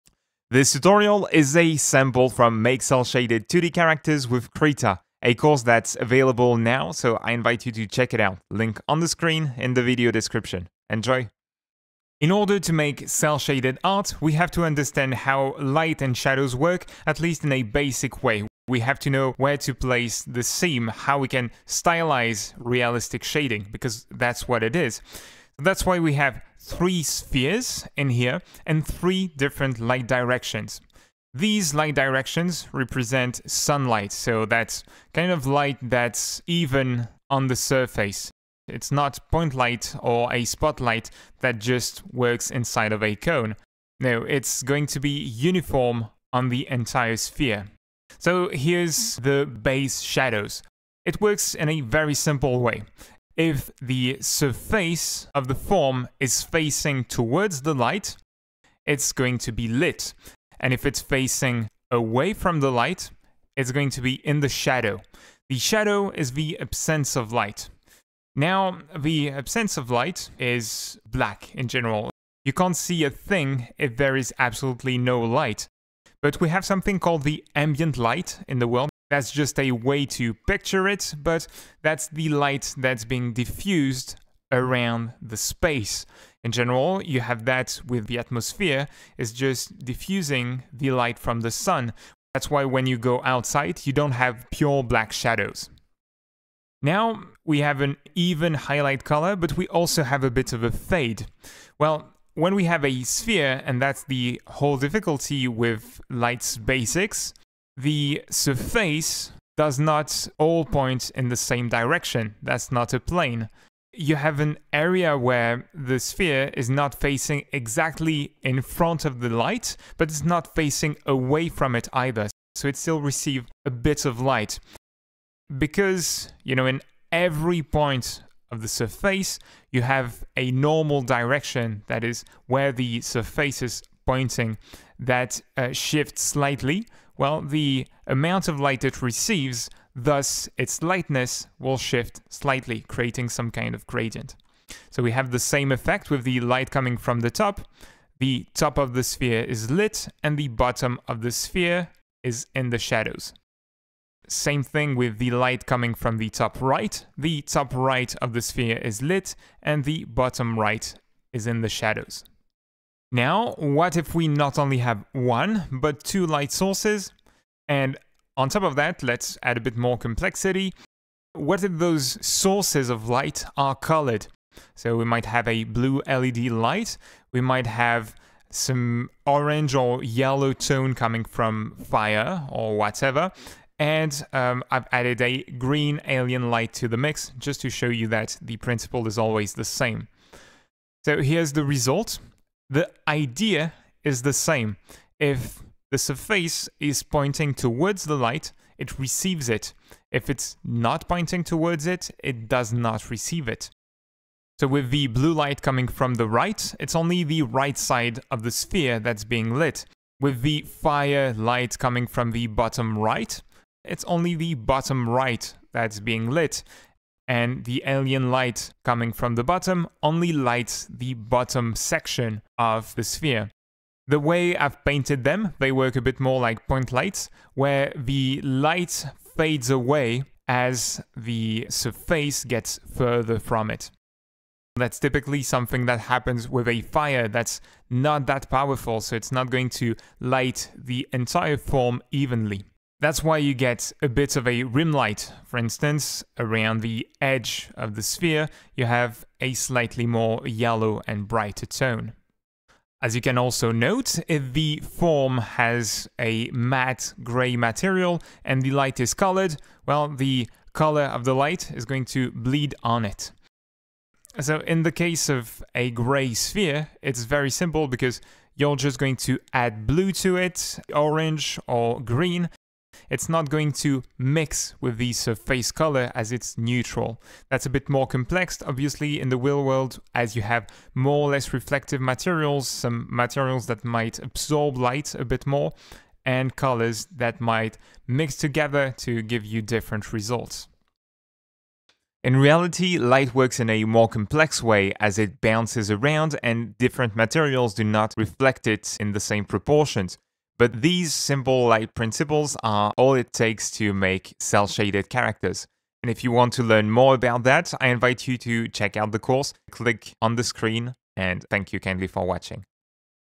Introduction to Lighting with Krita
Learn the basics of lighting for digital artists with Krita!
This is a sample from Make Cel Shaded 2d Game Art with Krita, a course that'll show you how to design characters based on an existing art direction (thanks to David Revoy's open webcomic Pepper and Carrot), but also to achieve a convincing cartoon art style!

This lighting tutorial is here to help you get started. Before we can simplify a character to get that cel shaded look, we need to understand how light and shadows work, at least in a simple way. The basics are: everything that's facing light is brighter, everything that faces away from light is in the shadow, and should be darker. The video will explain a bit more.